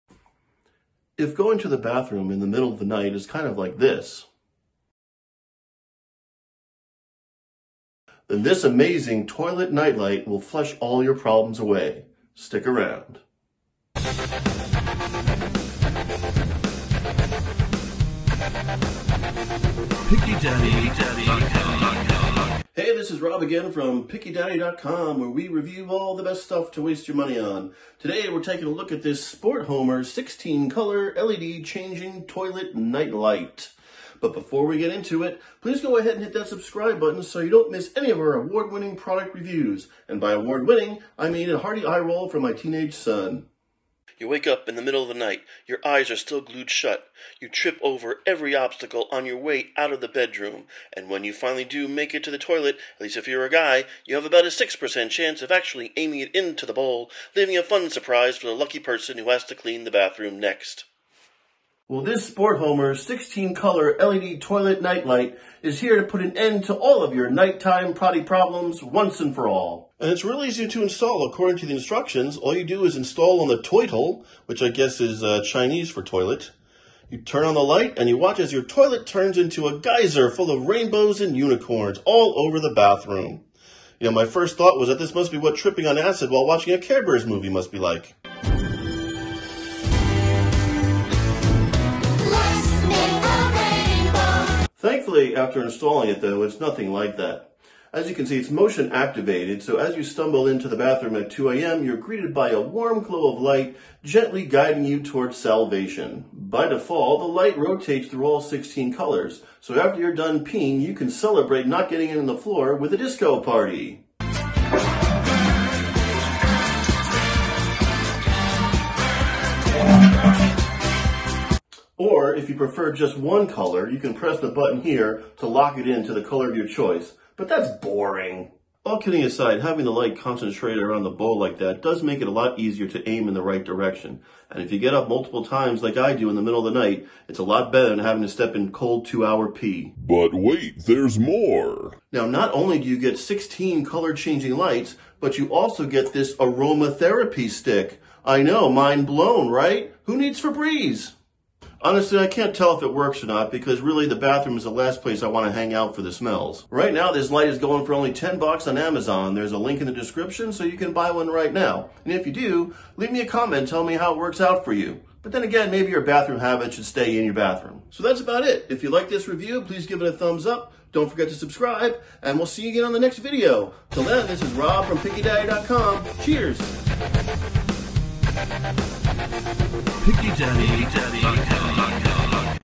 Best Color Changing Toilet Night Light Review!
This 16 color changing toilet night light is great for people who always get up in the middle of the night to pee.  With its soft glow and soothing rotating colors, you can be sure to aim correctly every time even when you're still half asleep.  The colors on this night light rotate or you can just choose to pick your 1 favorite color.

It's only $10 right now on Amazon